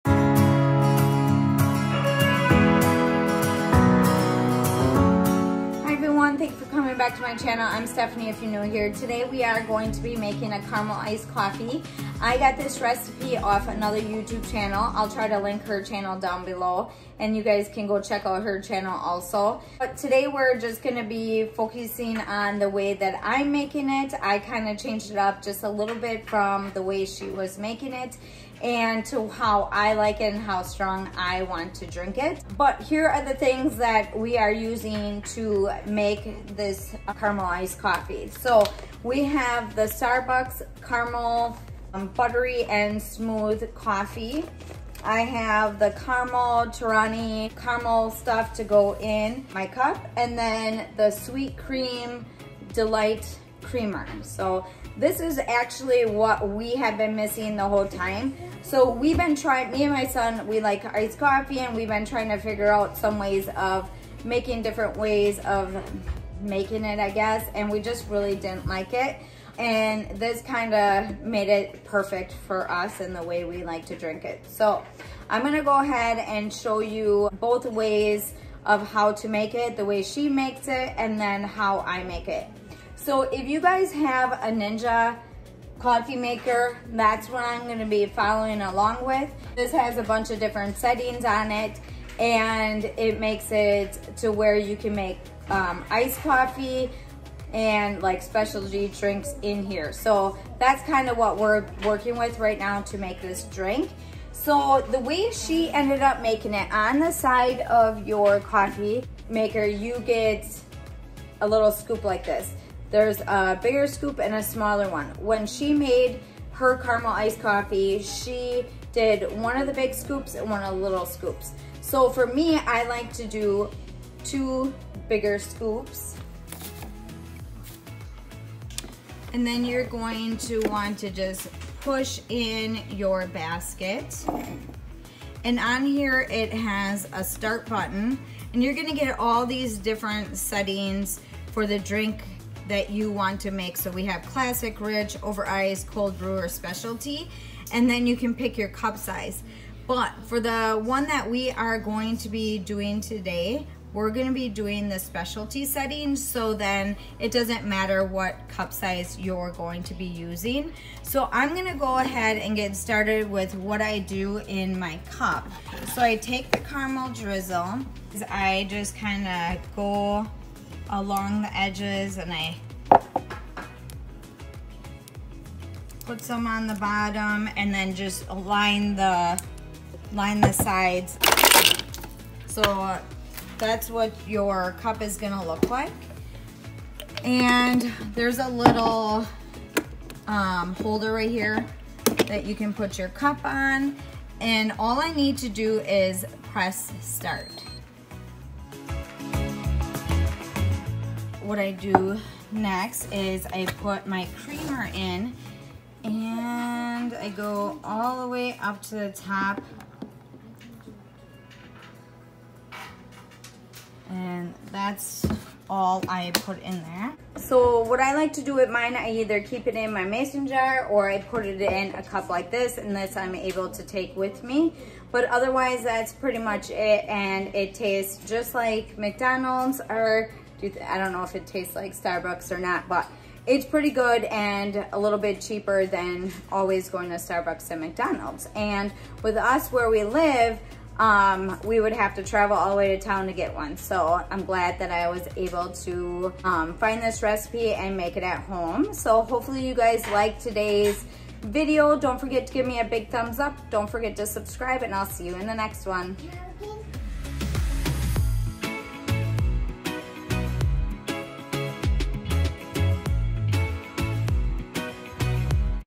The Best Caramel Iced Coffee Recipe | Ninja Coffee Maker | Mom of 10
The Best Caramel Iced Coffee Recipe
Starbucks buttery smooth coffee
Delight Creamer in Sweet Cream
ice
Mason jar
Torani Caramel sauce
Put the setting on Specialty Setting
Line your mason jar with caramel sauce
fill with ice
2 full scoops of Starbucks coffee in the filter
Press Start

➡️Silicone Mat
➡️Air Fryer accessories
➡️Air Fryer accessories